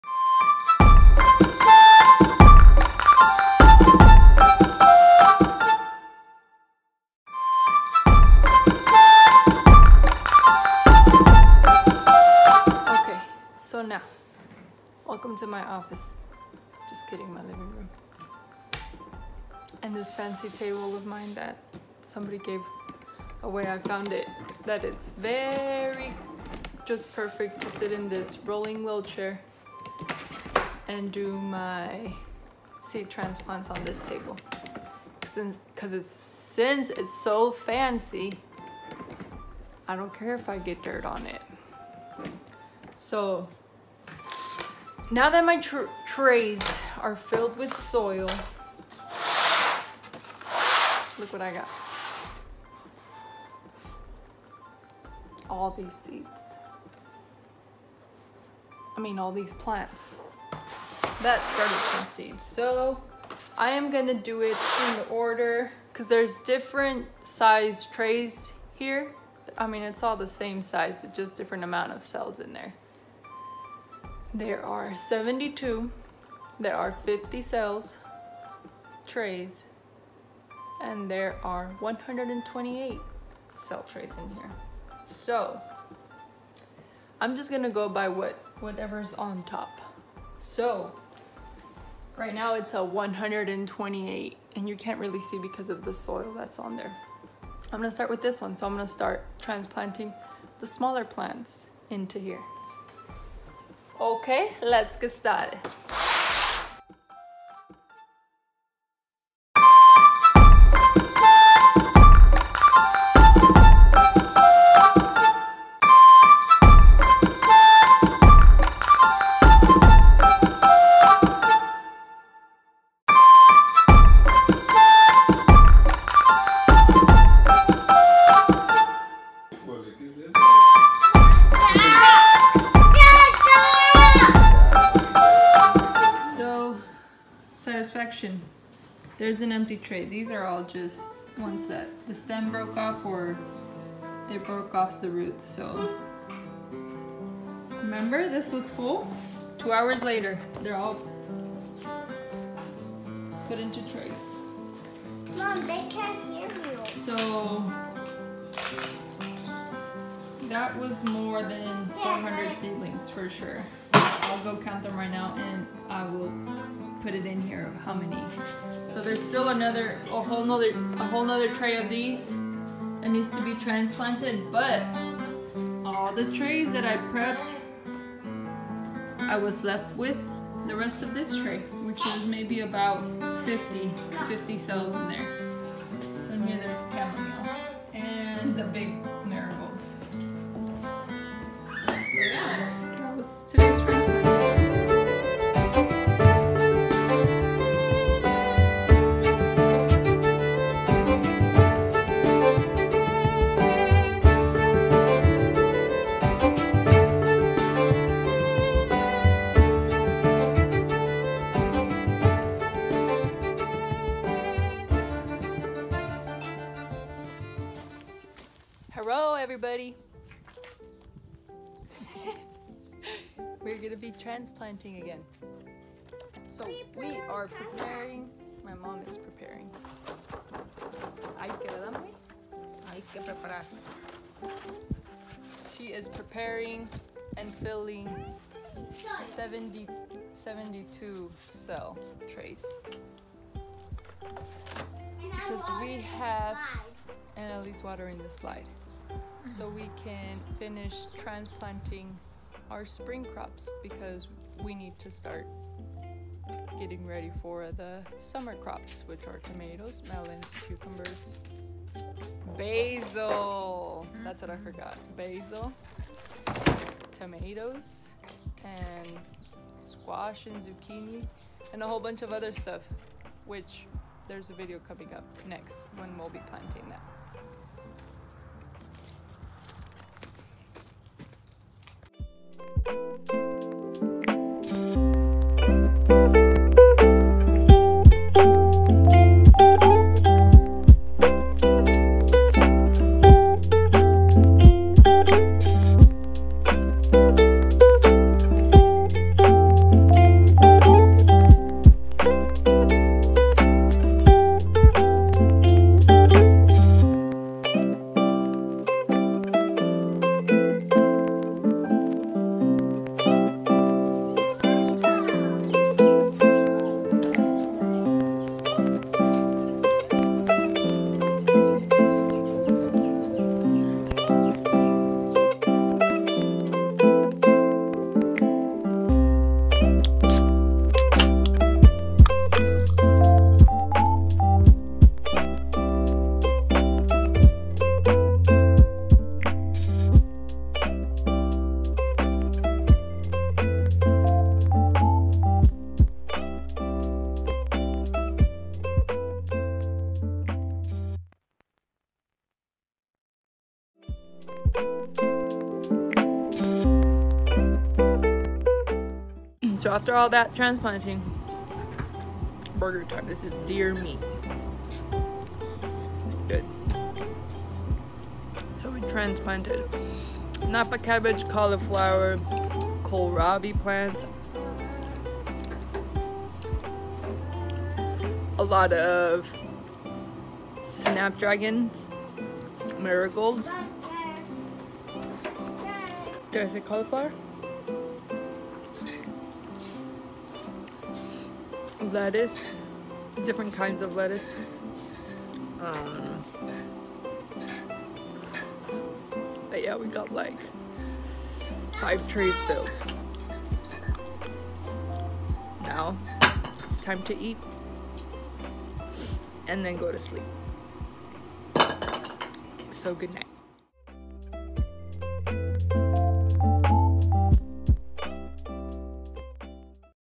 Transplanting 400+ seedlings
We transplanted the last of the spring seedlings, now we can start preparing for the summer crop seeds